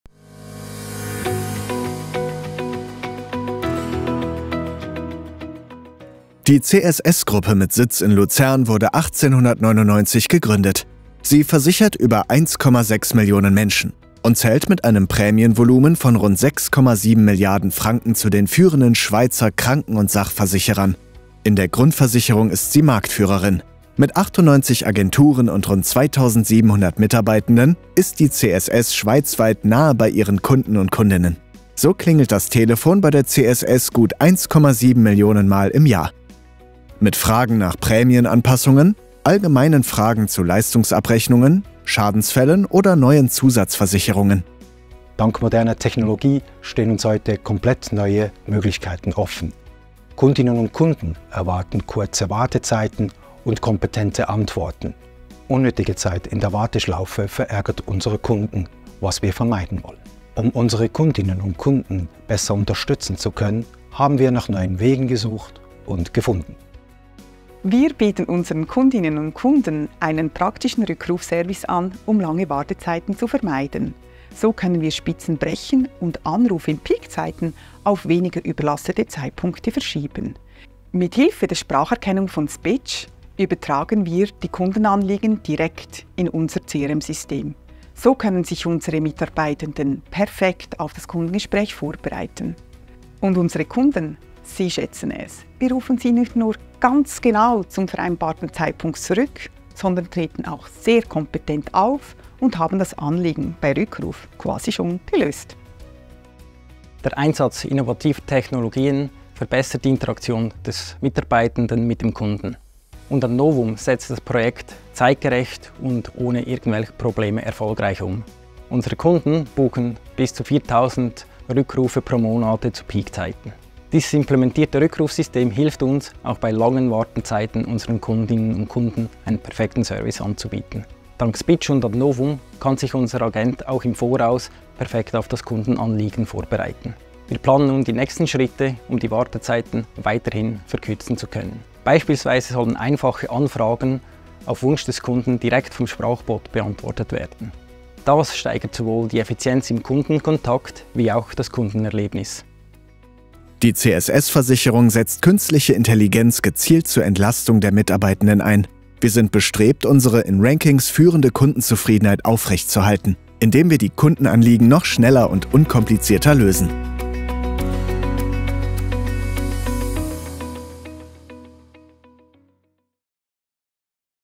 Conversational AI solution for the CSS Insurance contact center
To avoid long and unnecessary waiting times during peak hours, CSS tasked us with implementing an intelligent return-call solution for their contact center. This improves customer satisfaction and enables consultants to prepare for the next customer call, which in turn improves service quality.